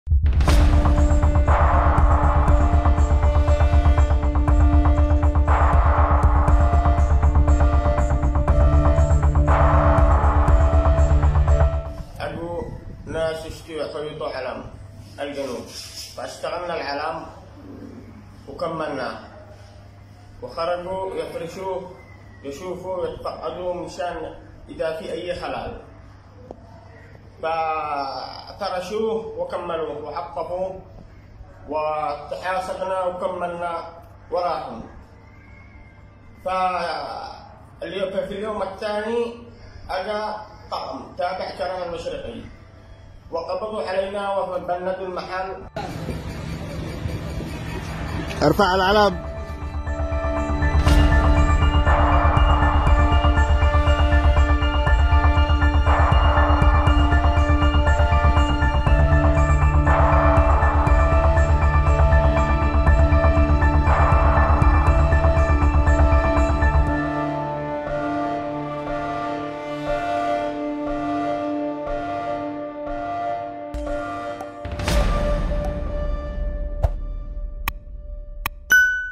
تهم ملفقة تتسبب بسجن عمّال خياطة لدى مليشيا الانتقالي بعدن.. شاهد القصة
"طلبوا منهم خياطة العلم الشطري وبعد أن أنهوه أُودعوا في السجن".. تهم ملفقة تجرّ عمّال خياطة إلى أحد سجون مليشيا الانتقالي في عدن
شاهد القصة